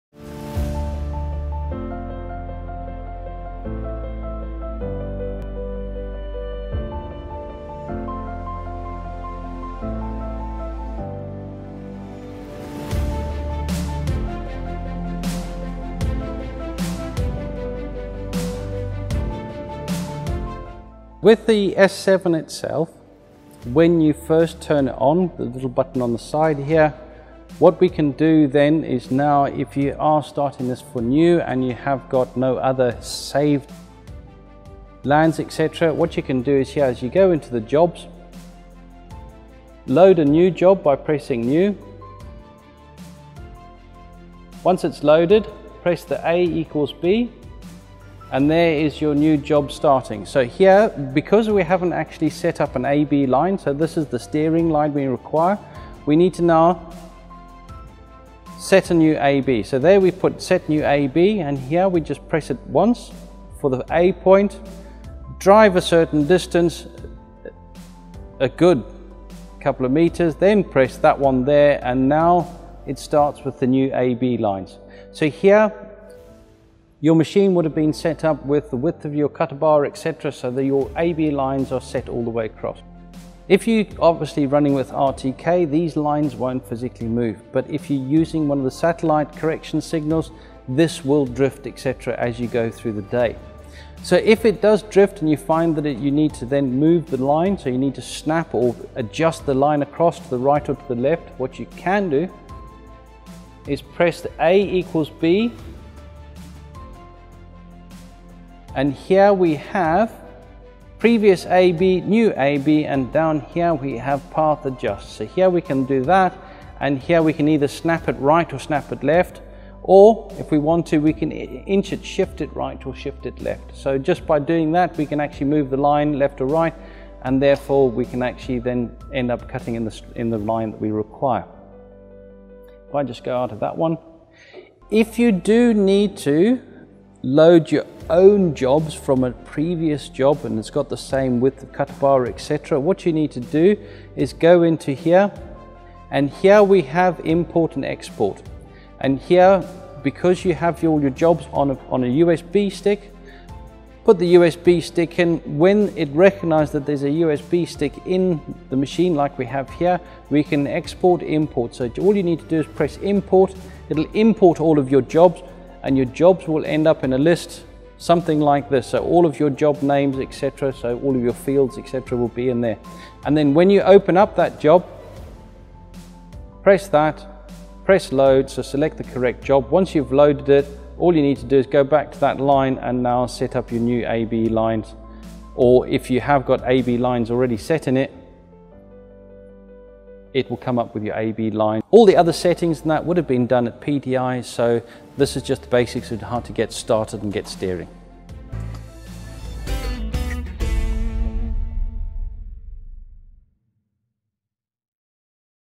TUCANO 400-300 Operators Tips - Automatics: Steering - GPS S7 terminal
This video takes a look at operation  of the S7 GPS Terminal for TUCANO 400 and TUCANO 300.